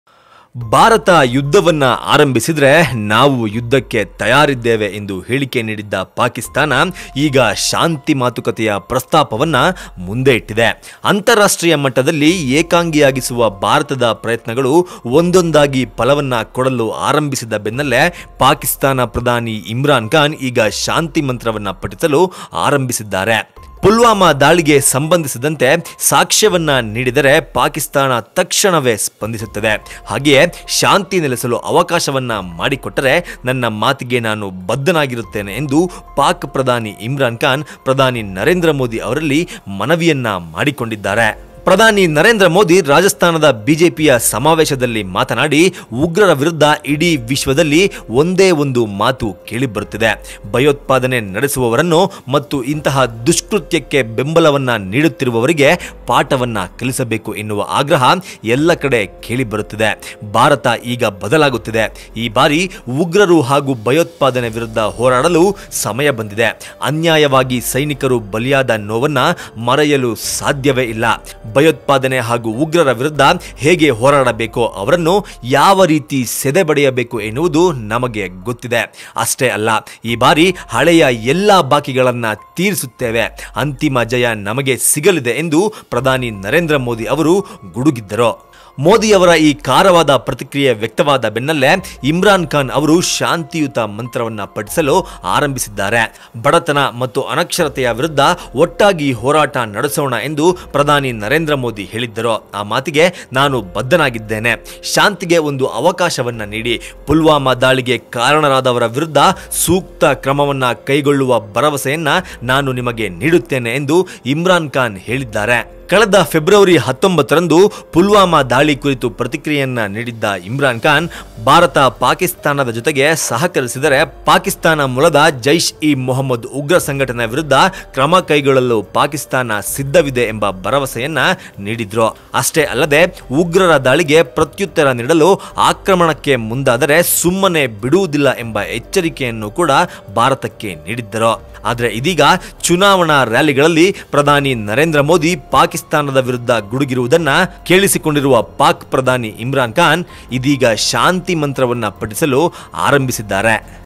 ಮೋದಿ ಅವಾಜ್ ಗೆ ಬೆದರಿದ ಪಾಕ್ ಶಾಂತಿ ಮಂತ್ರ ಪಟಿಸುತ್ತಿದೆ ಪಾಪ..! | Oneindia Kannada
ಭಾರತ ಯುದ್ಧ ಆರಂಭಿಸಿದರೆ ನಾವು ಯುದ್ಧಕ್ಕೆ ತಯಾರಿದ್ದೇವೆ ಎಂದು ಹೇಳಿಕೆ ನೀಡಿದ್ದ ಪಾಕಿಸ್ತಾನ ಈಗ ಶಾಂತಿ ಮಾತುಕತೆ ಪ್ರಸ್ತಾಪವನ್ನು ಇಟ್ಟಿದೆ. ಅಂತಾರಾಷ್ಟ್ರೀಯ ಮಟ್ಟದಲ್ಲಿ ಏಕಾಂಗಿಯಾಗಿಸುವ ಭಾರತದ ಪ್ರಯತ್ನಗಳು ಒಂದೊಂದಾಗಿ ಫಲ ಕೊಡಲಾರಂಭಿಸಿದ ಬೆನ್ನಲ್ಲೇ ಪಾಕ್ ಪ್ರಧಾನಿ ಇಮ್ರಾನ್ ಖಾನ್ ಈಗ ಶಾಂತಿಮಂತ್ರವನ್ನು ಪಠಿಸಲು ಆರಂಭಿಸಿದ್ದಾರೆ.

 Pak who has stated that if we start the war, we are ready for war, now has a peace talks. Pakistani Prime Minister Imran Khan has begun reciting Shanti Mantra after Modi statement.